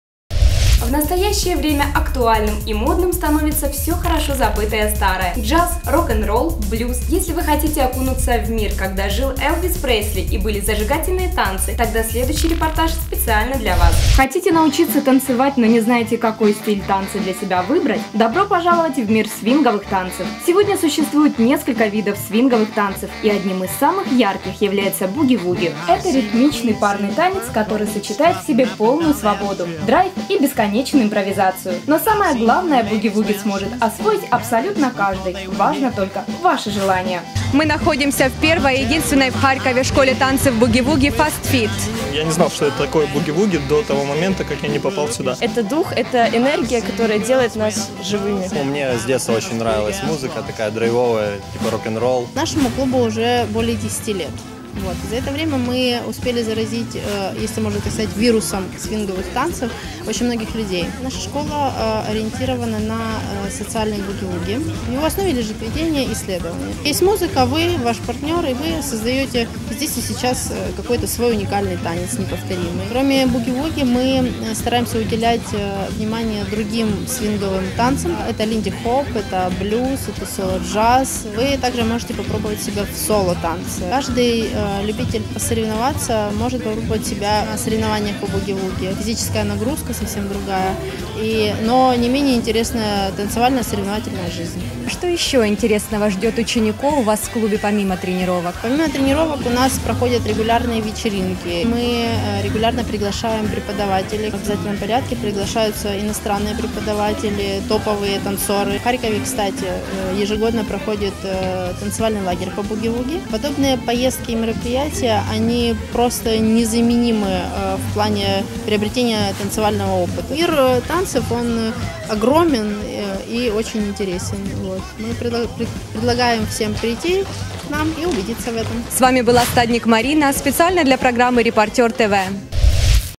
Fast Feet Club - единственная школа свинговых танцев в Харькове
Танцем под рок-н-ролл, джаз и блюз. Приходите, научим и вас :)